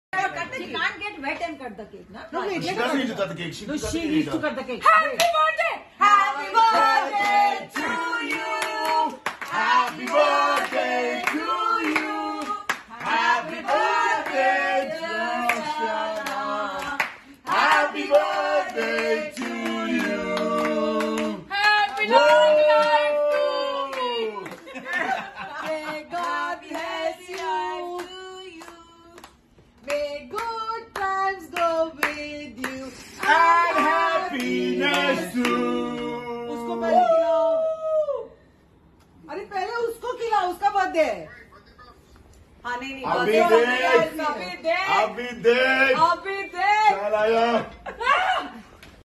ShehnaazGill celebrates her birthday with family and sidharthshukla 🎂❤
A dip that will go down for a good cause. This year shehnaazgill birthday is celebrated by her fans in a true spirit. Mostly all her fan clubs are doing some charity support. That's a great initiative especially by the North American fans clubs 🇺🇲🇨🇦

देखिये अपनी सासू माँ की कितनी इज़्ज़त करते है सिद्धार्थ| Sidharth Shukla Love For Shehnaaz Gill Mother

Sidharth की माँ ने इस तरह दिखाई Shehnaaz की माँ के लिए Respect, Video जीत लेगी आपका भी दिल |